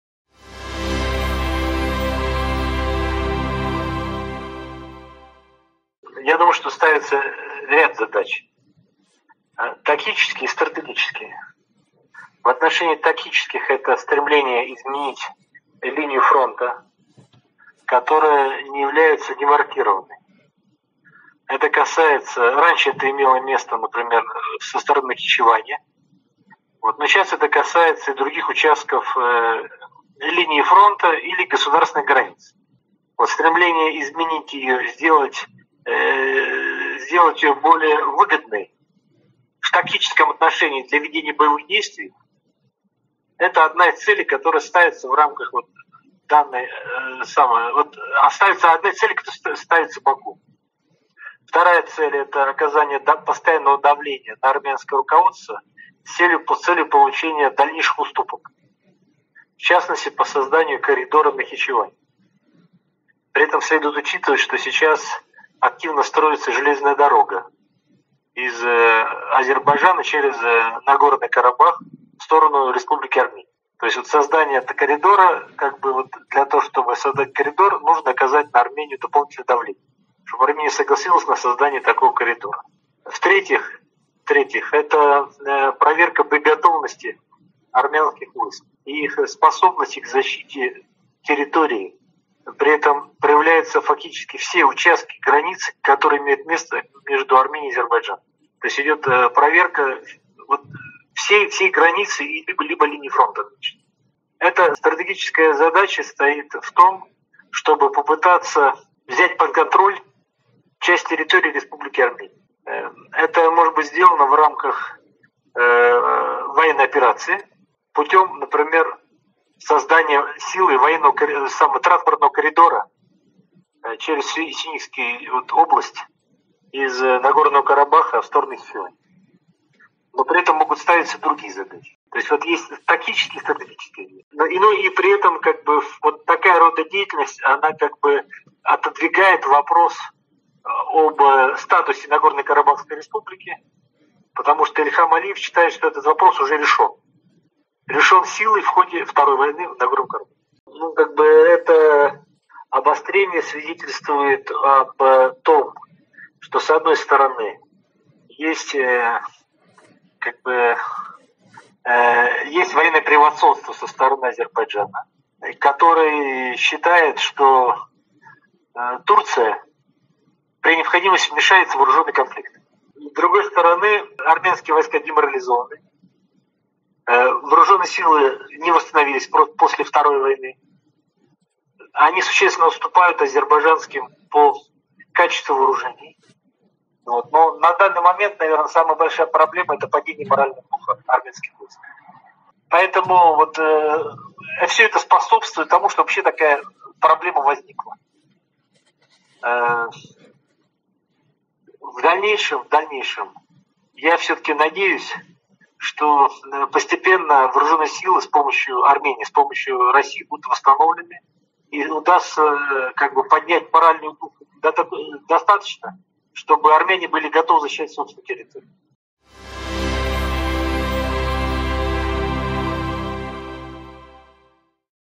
Какие задачи решает Баку, обостряя ситуацию на границе с Арменией: Евсеев
Российский военный эксперт Владимир Евсеев в беседе с корреспондентом РУСАРМИНФО прокомментировал обострение ситуации на границе Армении и Азербайджана.

По словам Евсеева, Баку поставил перед собой ряд задач – тактических и стратегических, выполнением которых сейчас занимается, обостряя ситуацию на границе.